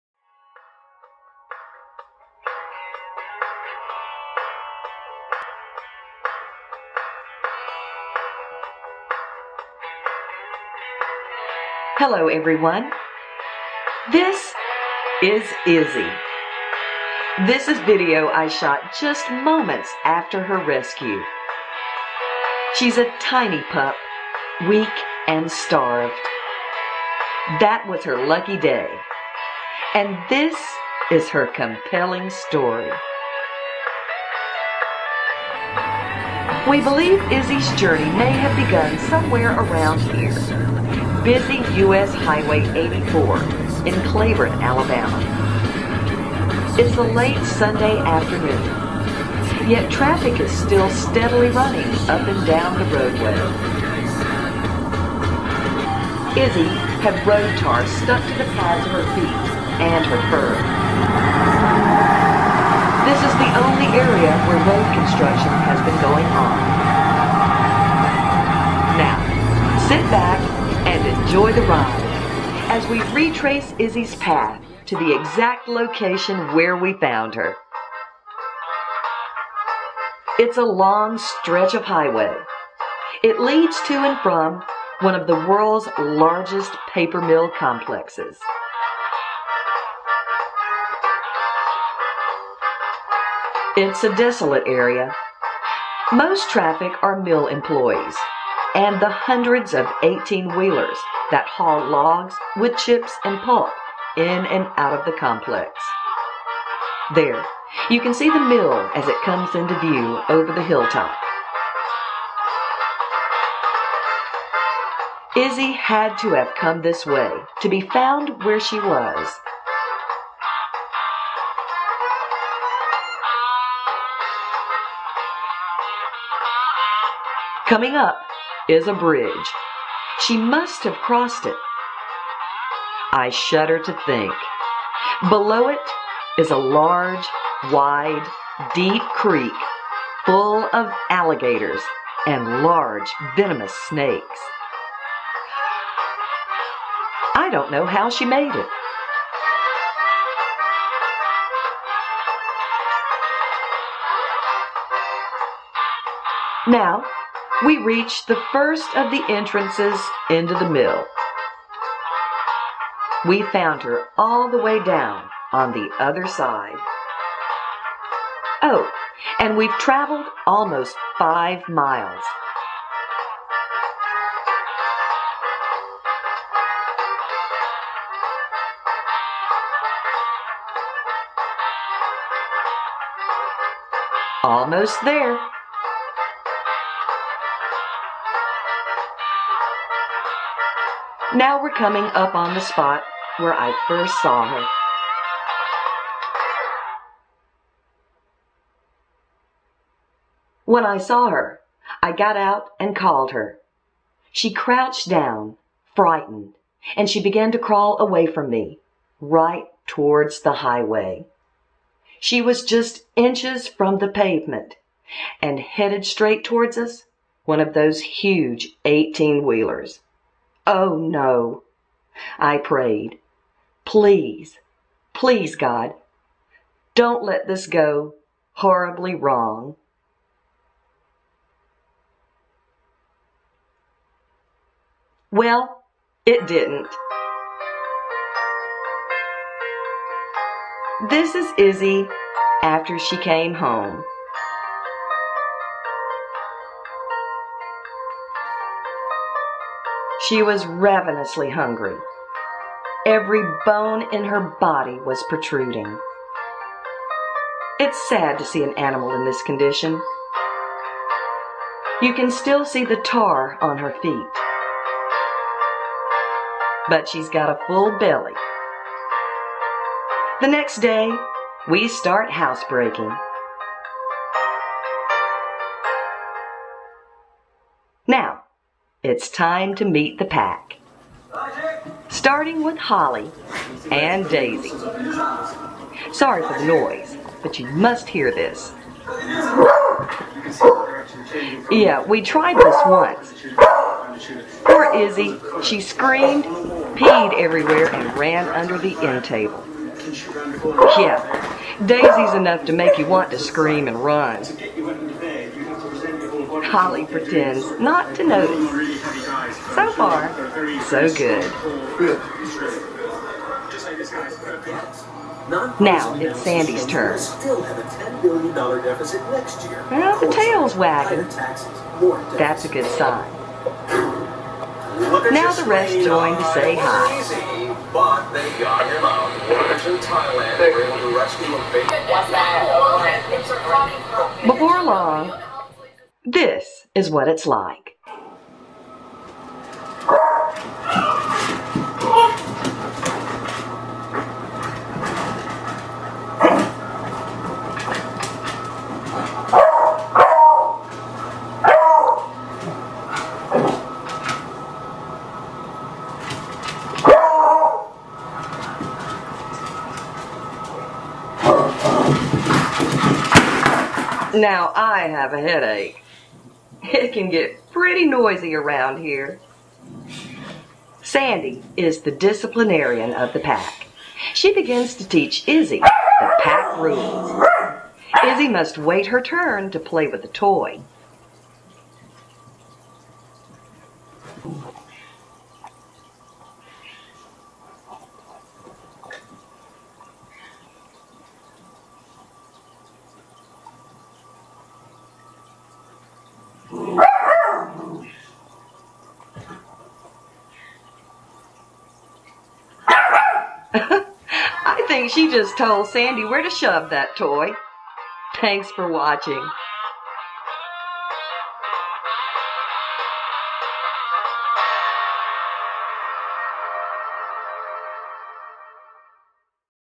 This is Izzy.
Meet our latest rescue dog. Help us continue to care for these animals and give them the second chance at the good life they deserve. Thank you for your support.:)